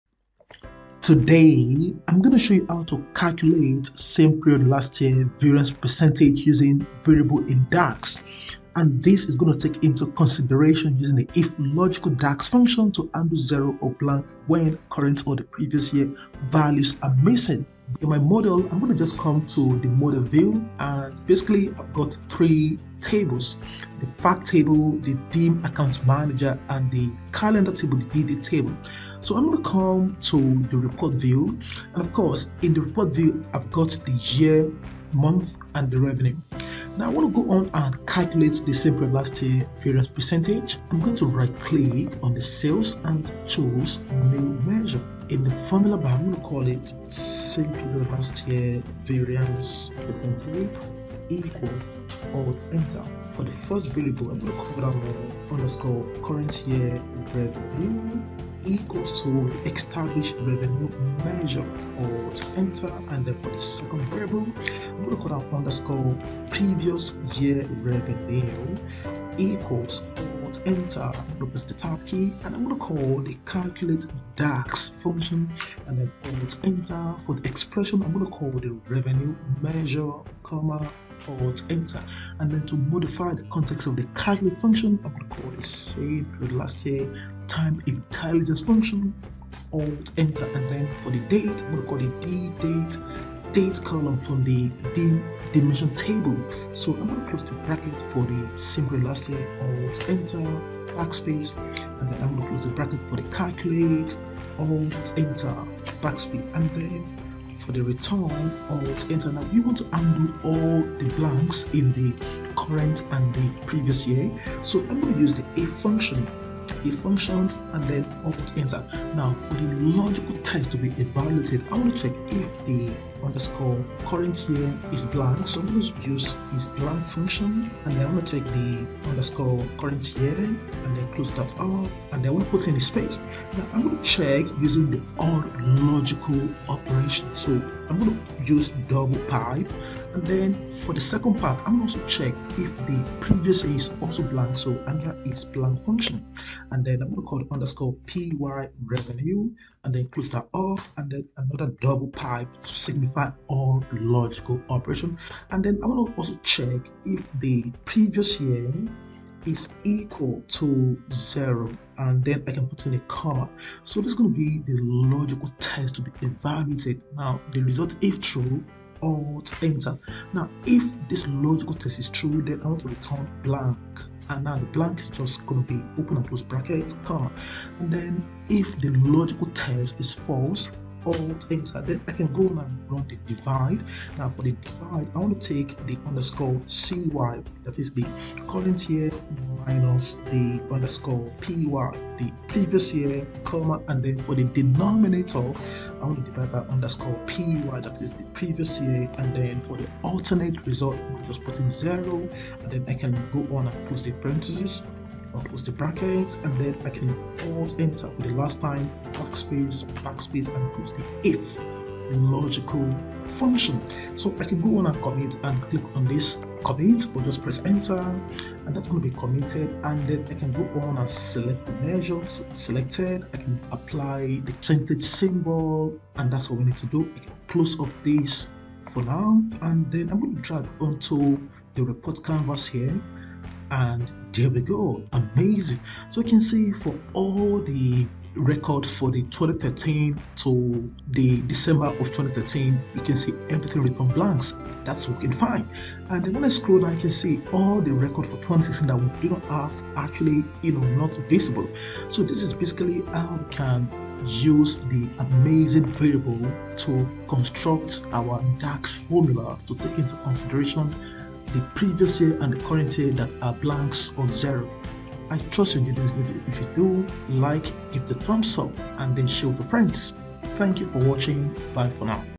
How to Calculate SPLY Var (%) Properly in Power BI using Variables in DAX
This video shows How to Calculate SPLY Var (%) Properly in Power BI using Variables in DAX